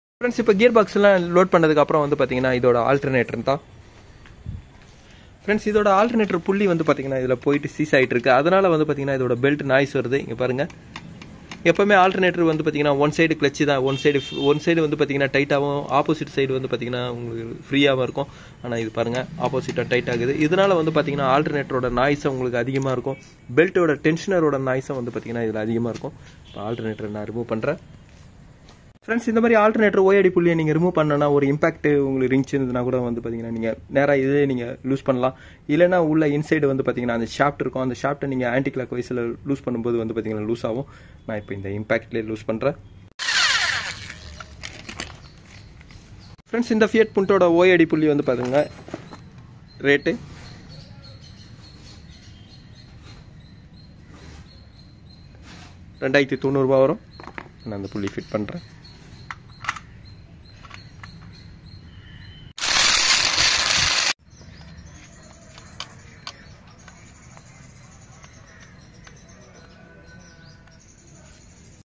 Fiat alternator pulley change
Belt noise complaint alternator oad pulley change and price (@skilledmechanictamil )